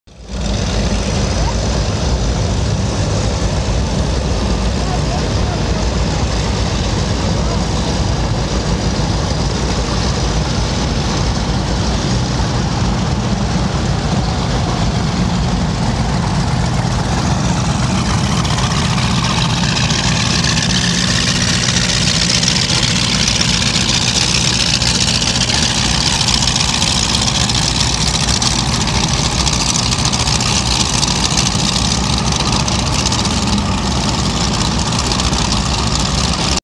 Boyne Thunder Heading Through the Pine River Channel
You can get a feel for the power of these high performance boats as they motor down the Pine River Channel in Charlevoix, MI.